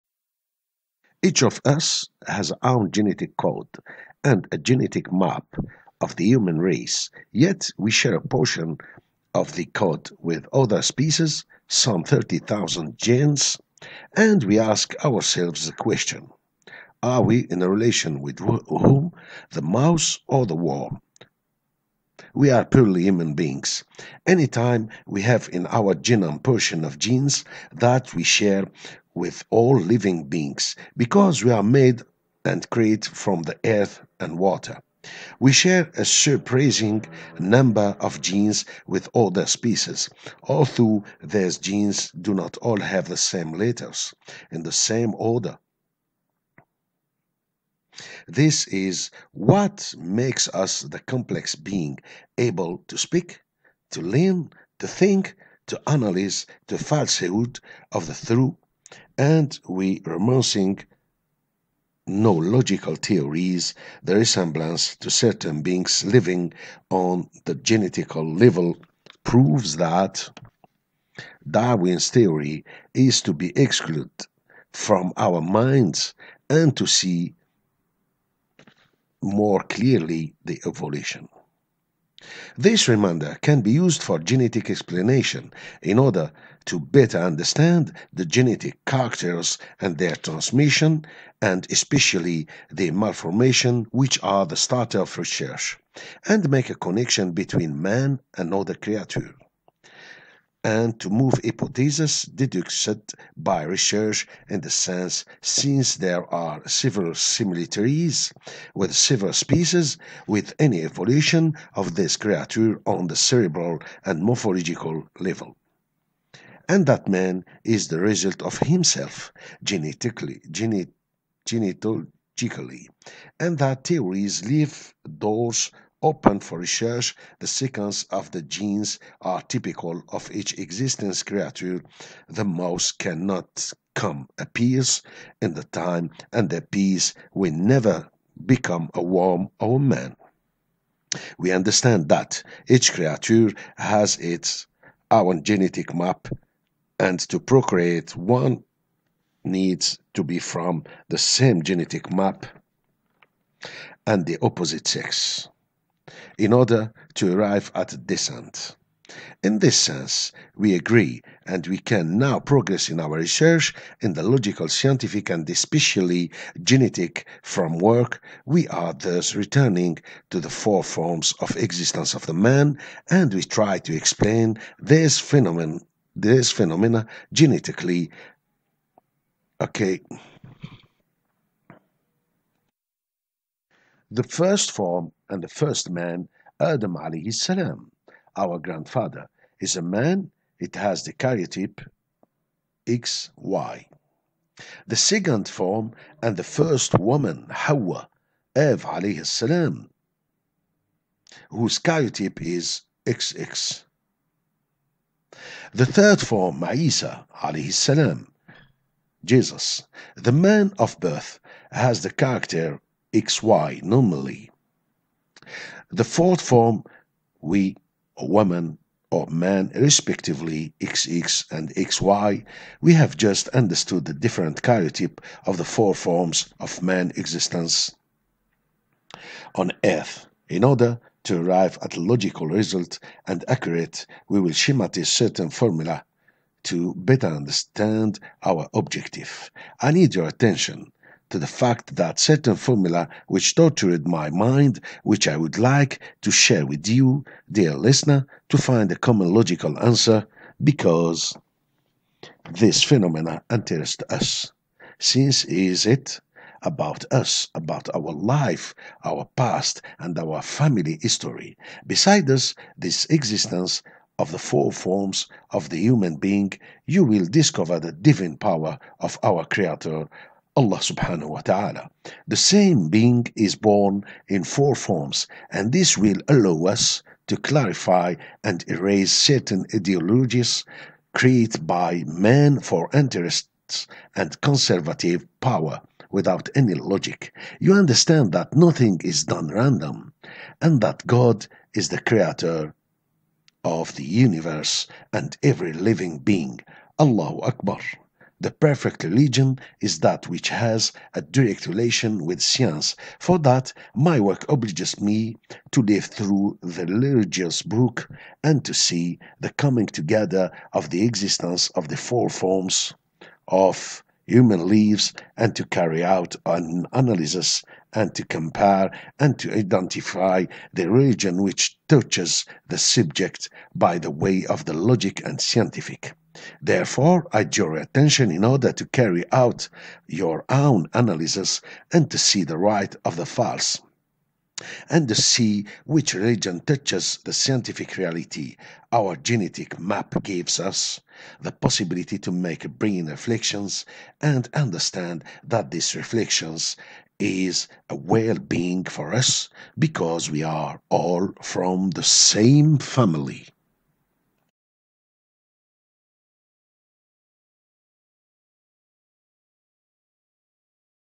The four forms of our existence video 5
-Likeness with other beings
-The genetic map
- The karyotypes of the four forms
- Our big family